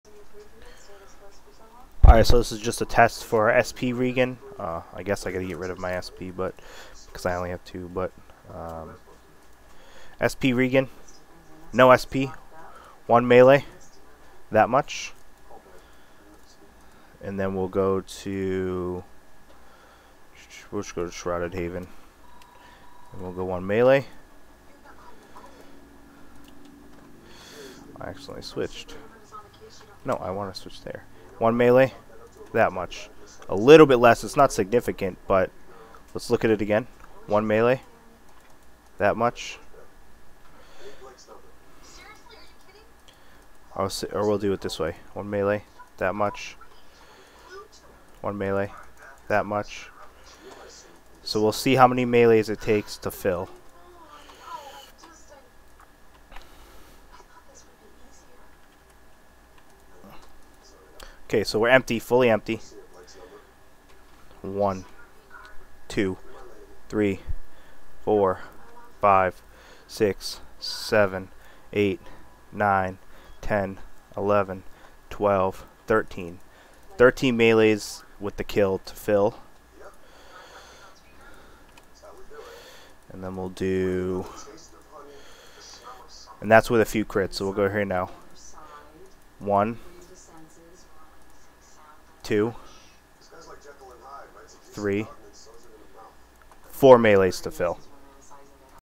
SP regen Test
Testing how damage effects SP gain.